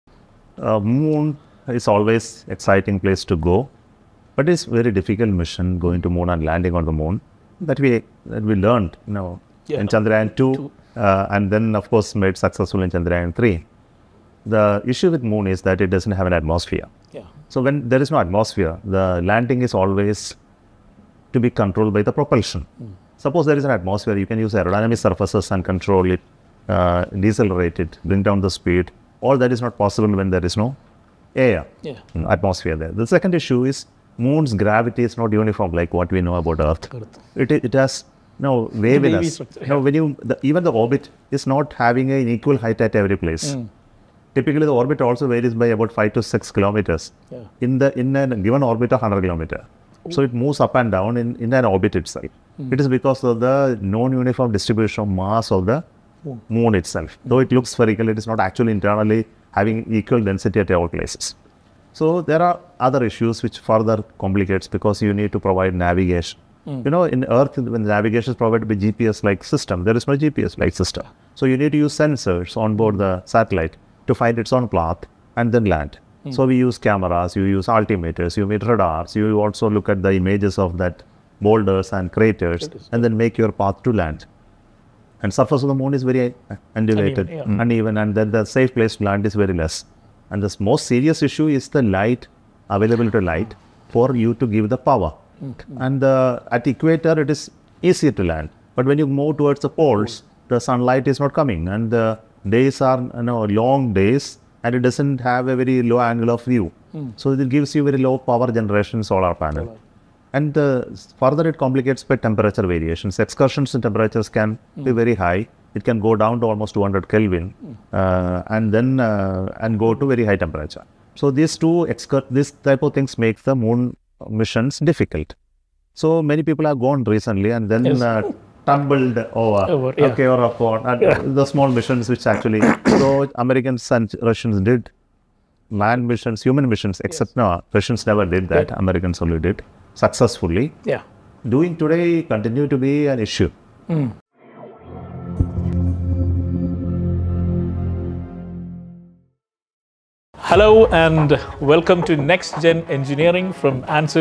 Why Reaching the Moon Is Still a Challenge
It looks so close in the night sky—but the Moon is one of the toughest places to reach. From escaping Earth’s gravity to surviving deep space travel, from landing with precision on rough lunar terrain to keeping spacecraft safe in extreme heat and cold—every step is a test of engineering and endurance.
In this video, we explore why reaching the Moon remains a challenge even today, and why every successful mission is a triumph of science, technology, and human spirit.